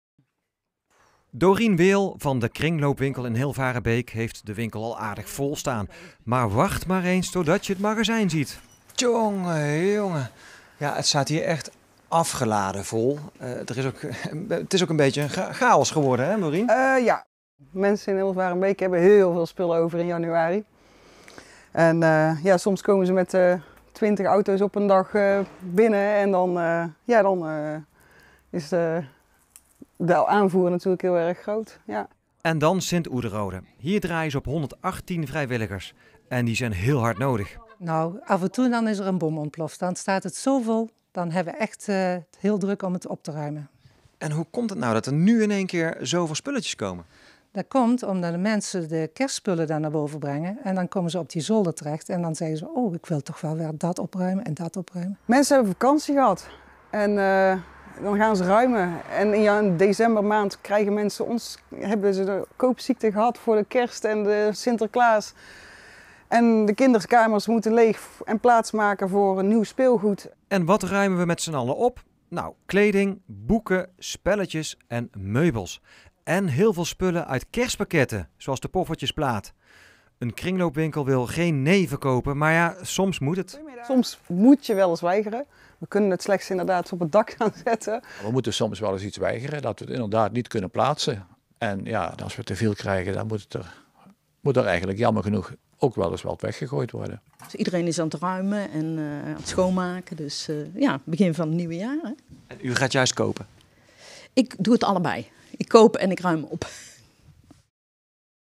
Opruimwoede: Brabantse kringloopwinkels worden overspoeld met spullen
Kringloopwinkels in Brabant komen om in de spullen. Ze weten soms niet meer waar ze het moeten laten. Iedereen is tijdens de vakantie aan het opruimen geslagen en daardoor worden de winkels overspoeld met spullen.